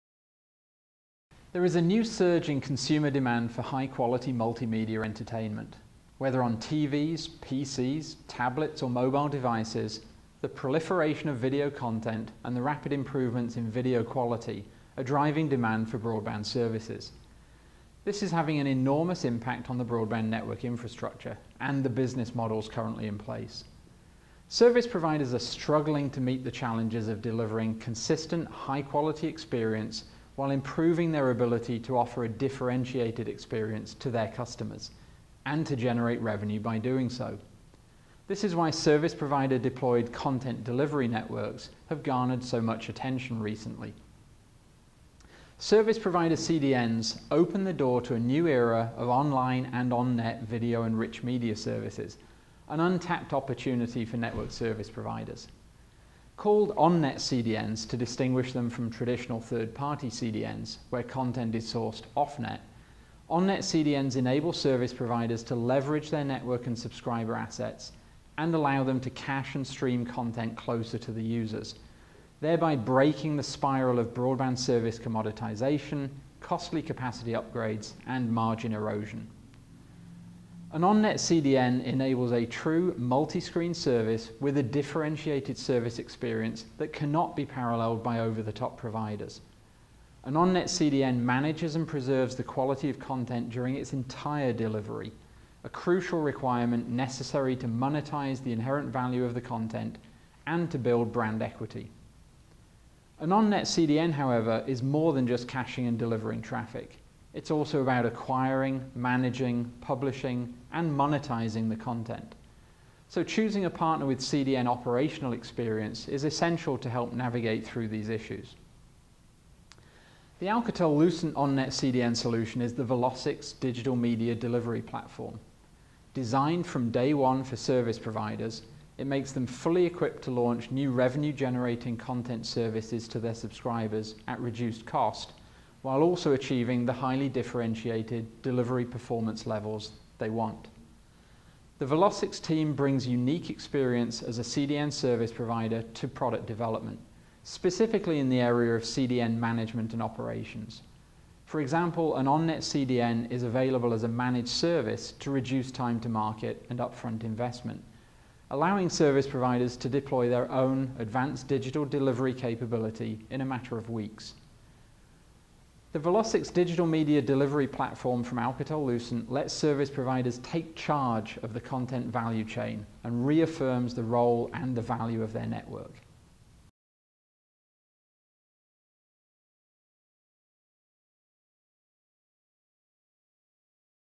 Alcatel-Lucent -- On-Net Content Delivery Networks
Should Service Providers develop and deploy their own content delivery networks? Here's a two-minute look at how Service Providers can leverage on-net CDNs to break the cycle of broadband commoditization. Presented by Alcatel-Lucent's Lindsay Newell.